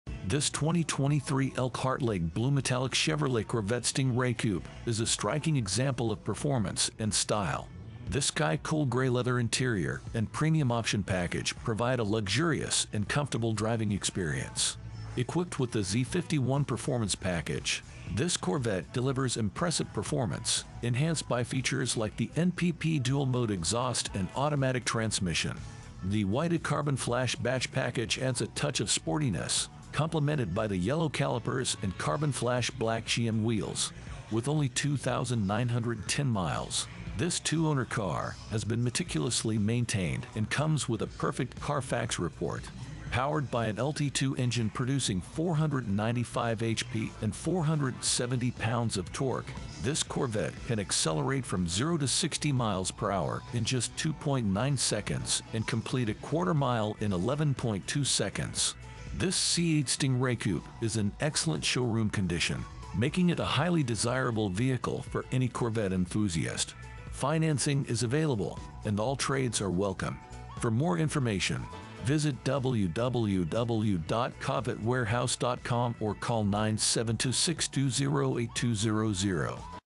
2023 Chevrolet Corvette Elkhart Lake Blue Metallic Coupe Z51
2023 Elkhart Lake Blue Metallic / Sky Cool Gray Leather Chevrolet Corvette Stingray Coupe Premium Option Package, Z51 Performance Package, EYT Carbon Flash Badge Package, NPP (Dual Mode Exhaust), Automatic Transmission, Removable Top, Yellow Calipers, and the Carbon Flash Black GM Wheels with only 2k Miles!

Two-Owner / Perfect Carfax!

LT2/495hp

Quarter-mile in just 11.2 seconds!

470 Pounds of Torque!

This is a gorgeous color with only 2,910 miles!

This C8 Stingray Coupe comes equipped with the Premium Option Package, Z51 Performance Package, EYT Carbon Flash Badge Package, NPP (Dual Mode Exhaust), power windows, power locks, power mirrors, keyless entry, removable top, yellow calipers, and the Carbon Flash Black GM Wheels. It is a two-owner Corvette in excellent showroom condition and has been highly maintained. This 2023 Mid-Engine Stingray is brand new out of the wrapper!!

FINANCING AVAILABLE / ALL TRADES WELCOME